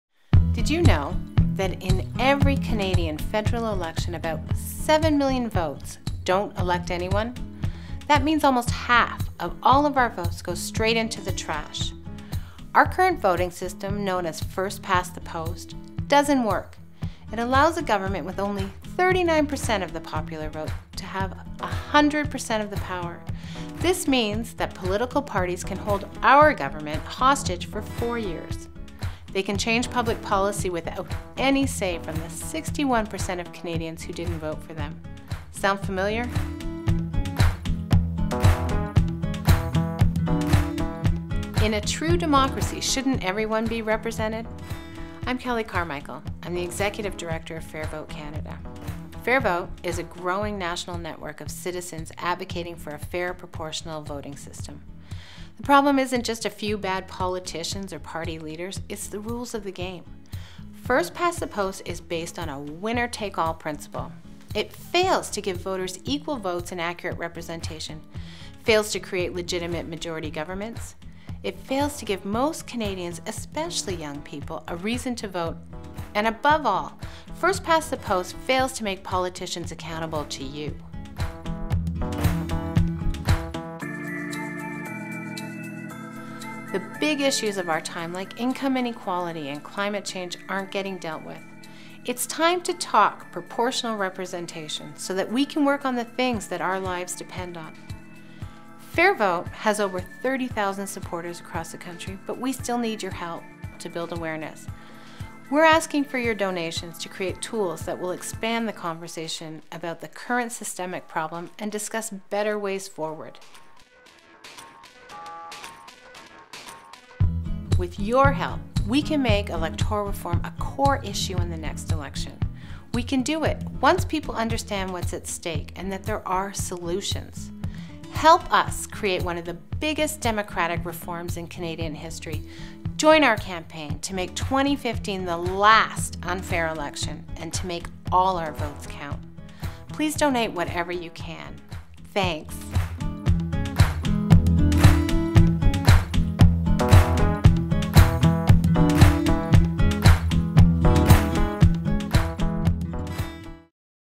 Help Make 2015 The Last Unfair Election
We are raising funds for our 2015 Campaign! Our goal is to build one of the biggest democratic reform movements in Canada's History. Help us Make 2015 the Last Unfair Election!